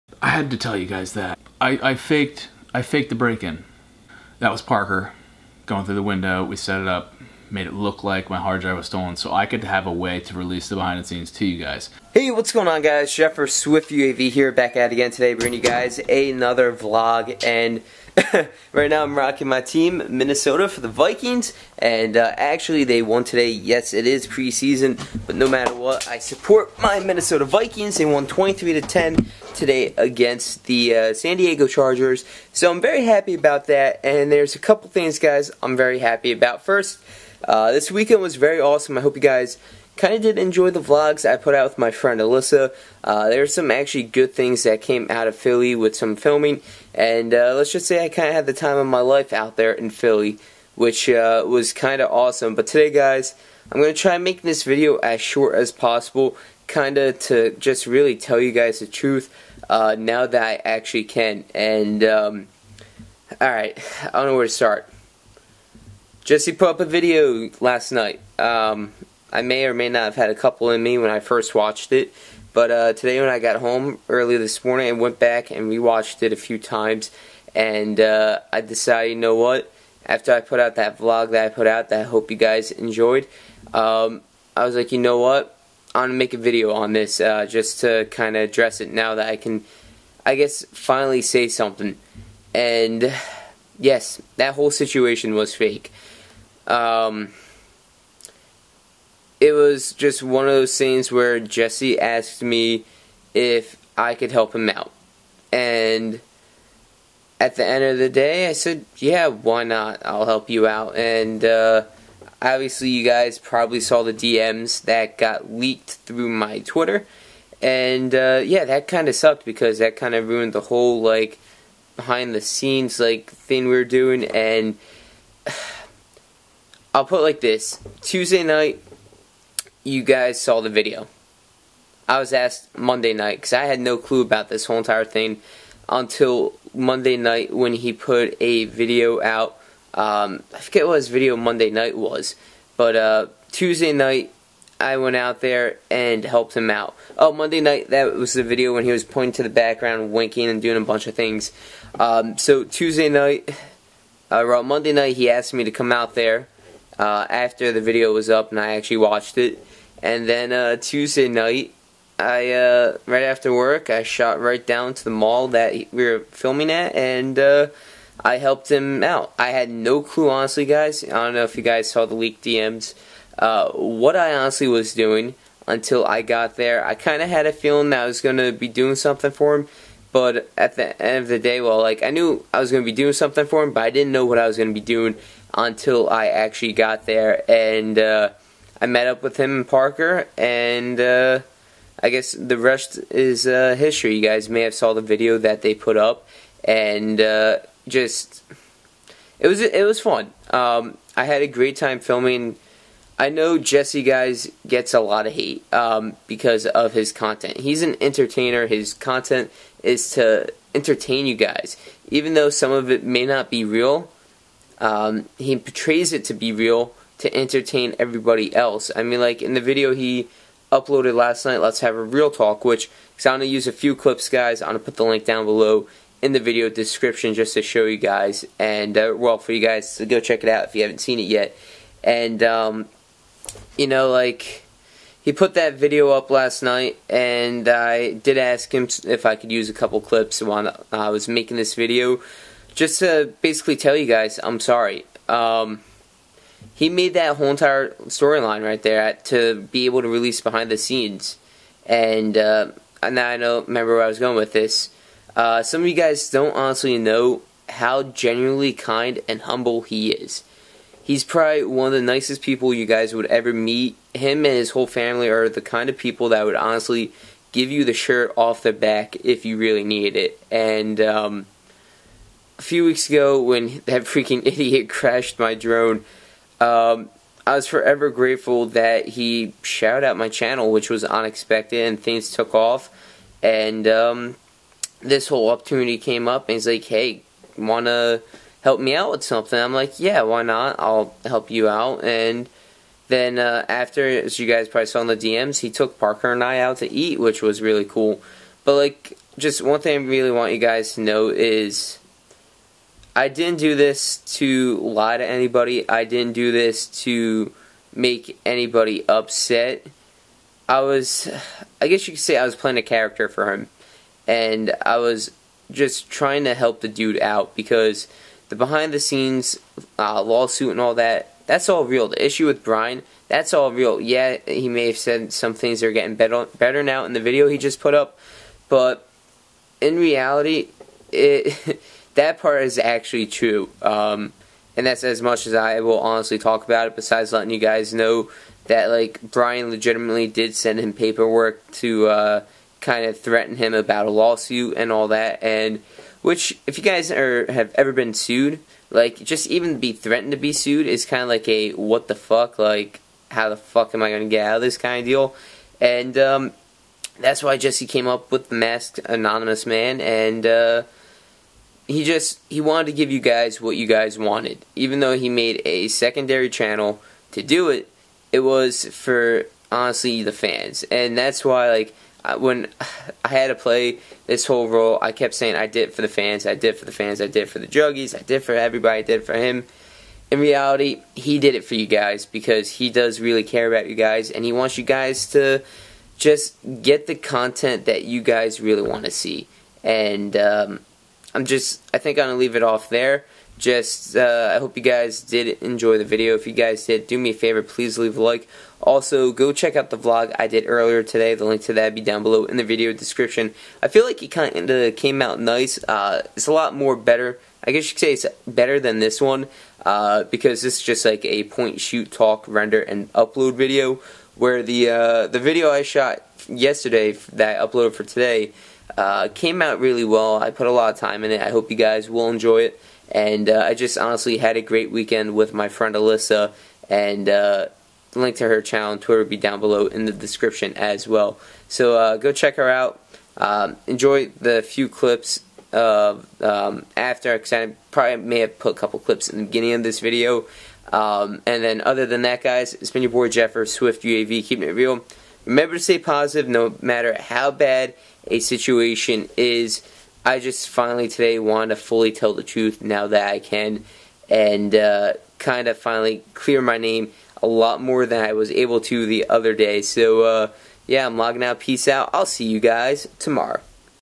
Clears My Name? Telling You The Real Truth!
Well McJuggerNuggets made a new video last night called "LET'S HAVE REAL TALK!" and informs you of the BTS Situation. Now are you ready for the truth from SwiftUAV?

-Connect With SwiftUAV-
► New Videos Everyday!